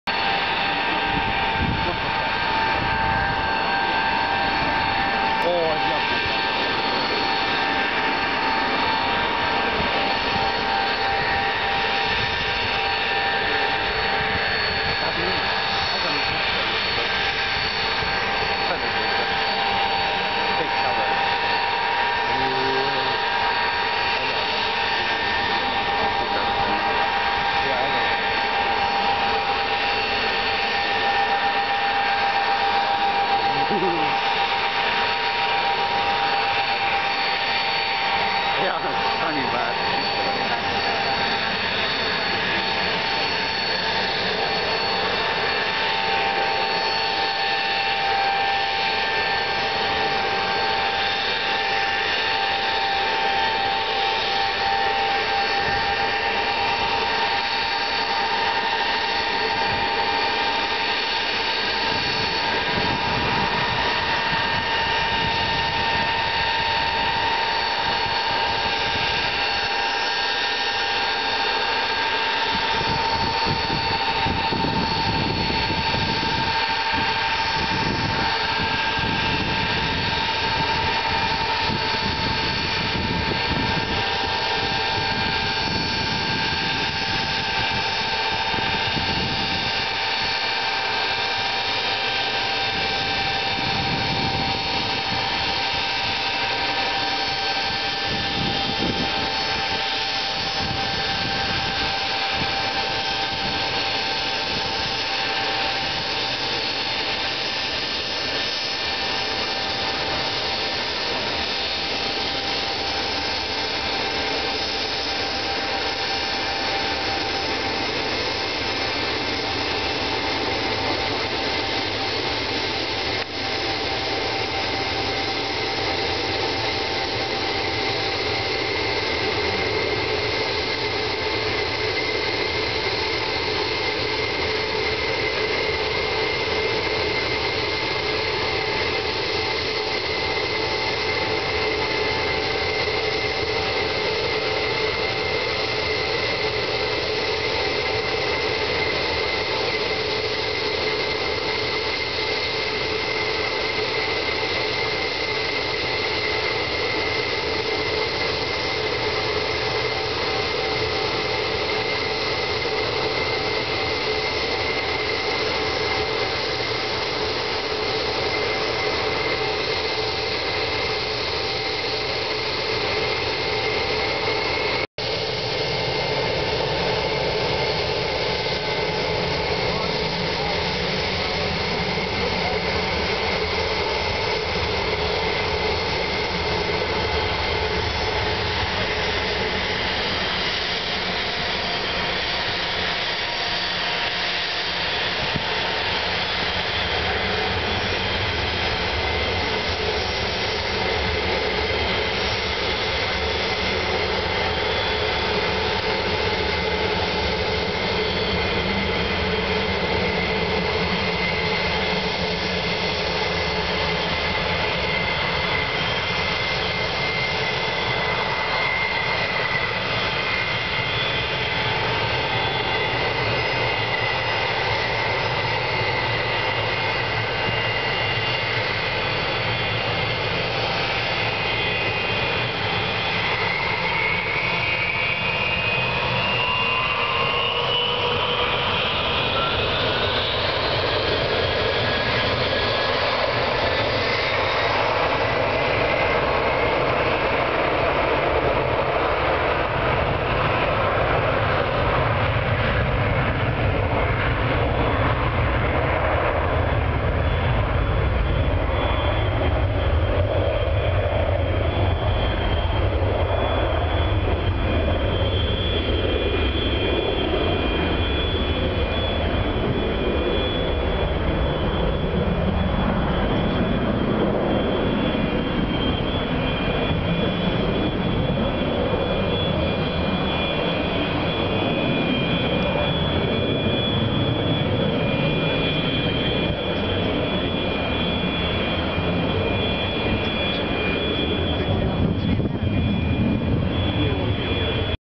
Tu-154M taxi and takeoff at Ferihegy 13R
Tu-154M taxi and takeoff at Ferihegy 13R. (Cargo mound.)
Tu-154M gurulás és felszállás Ferihegy reptér 13R-ről.
(Videó a Cargo dombról.)